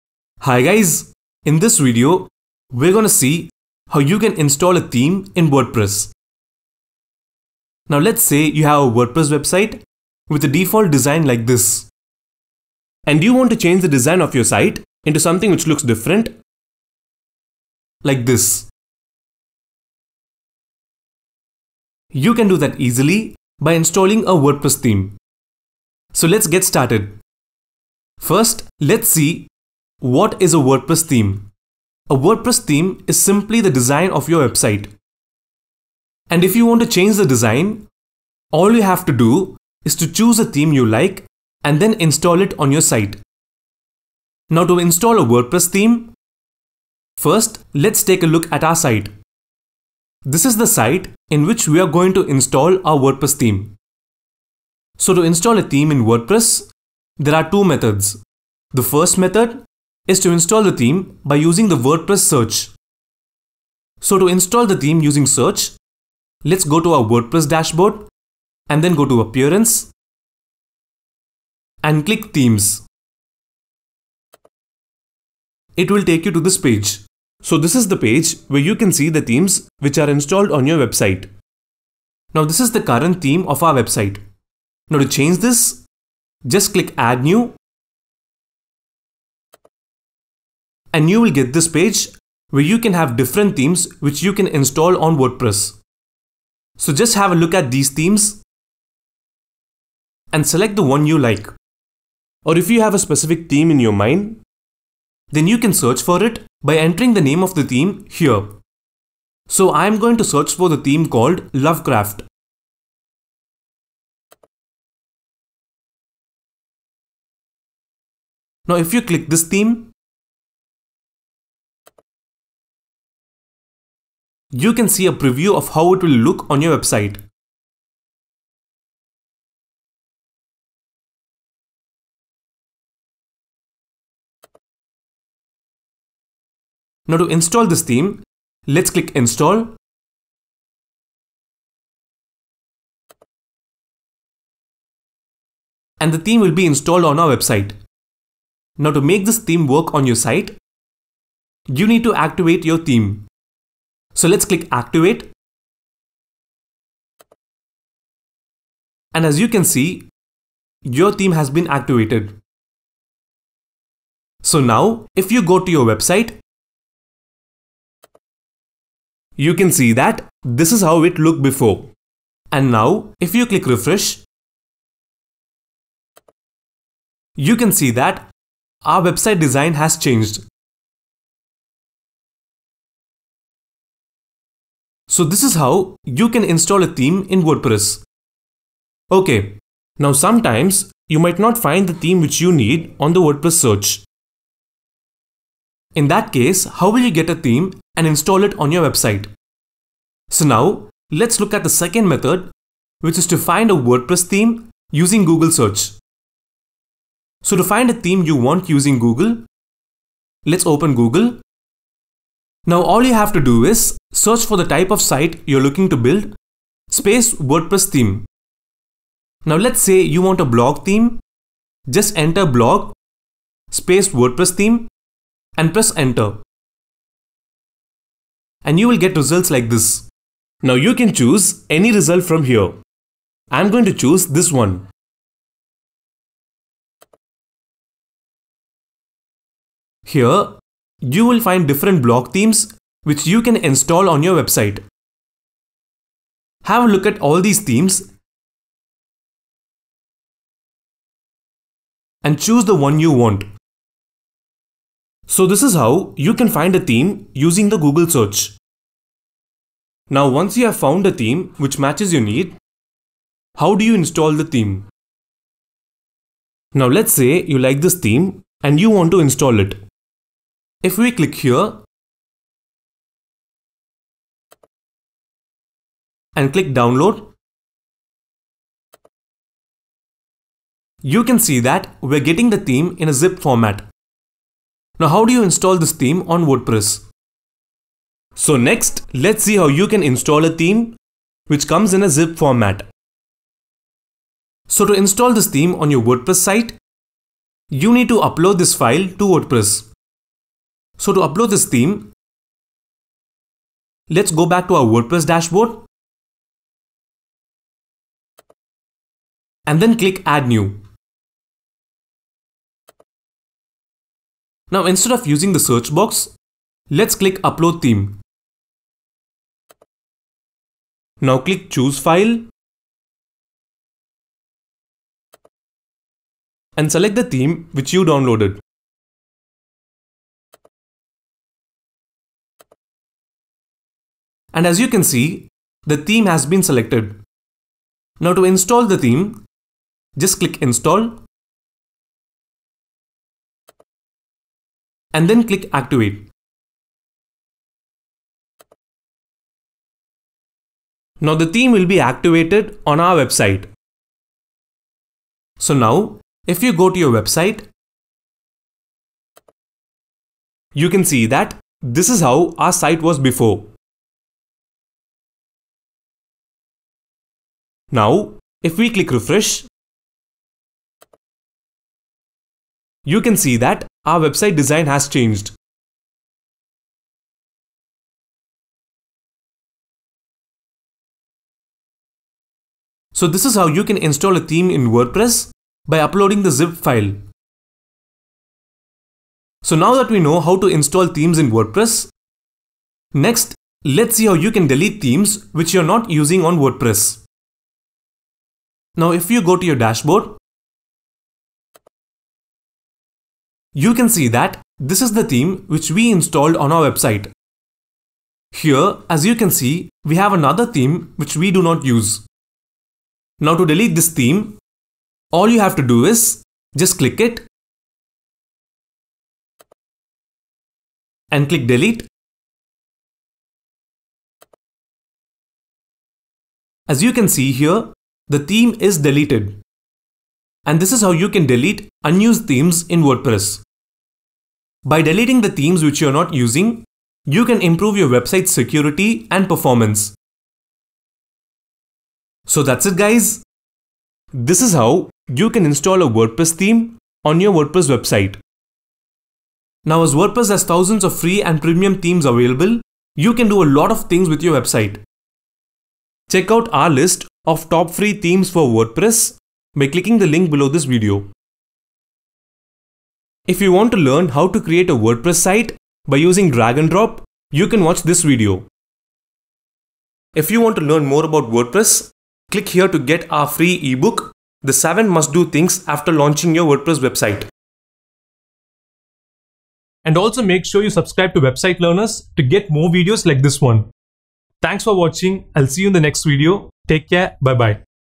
How to Install a WordPress Theme
Learn how to install a theme in WordPress

0:00 Intro
0:44 To install a WordPress theme
0:58 Install the theme by using WordPress search
3:20 Find a WordPress theme using Google search
4:48 How to install a theme in a zip format
6:33 How you can delete themes on WordPress

To install a theme, first you need to find a theme.
You have 2 methods to install a theme

1. Using WordPress search

2. Using Google search

First method in installing a theme is by using WordPress search.
Go to your dashboard ► Appearance ► Themes
So, to add a theme, click ‘Add New’.
You can see a number of themes which can be installed on WordPress.
To search for a particular theme,
Enter the theme name in the search box
Now click ‘Install’ & click ‘Activate’.

To find a theme using google search.
Search for the theme on Google
Now have a look at all these results and choose your theme.
To install it, download the theme.
The downloaded theme will be in zip format.
Now you need to upload this theme in WordPress by clicking ‘Add New’ in the theme page.
Click ‘Upload Theme’ &  choose your zip file.
Once you’ve uploaded the theme, click ‘Install & Activate’.
So now the theme will be installed on your WordPress website.

This is how you can install a theme in WordPress.

🔥 𝗪𝗲𝗯𝘀𝗶𝘁𝗲 𝗚𝘂𝗶𝗱𝗲 - 𝗦𝗶𝗺𝗽𝗹𝗲 & 𝗘𝗮𝘀𝘆 𝗦𝘁𝗲𝗽𝘀 𝘁𝗼 𝗹𝗮𝘂𝗻𝗰𝗵 𝘆𝗼𝘂𝗿 𝗳𝗶𝗿𝘀𝘁 𝗪𝗲𝗯𝘀𝗶𝘁𝗲

🎓 𝗥𝗲𝘀𝗼𝘂𝗿𝗰𝗲𝘀 𝘁𝗼 𝗴𝗲𝘁 𝗯𝗲𝘁𝘁𝗲𝗿 𝗮𝘁 𝗪𝗼𝗿𝗱𝗣𝗿𝗲𝘀𝘀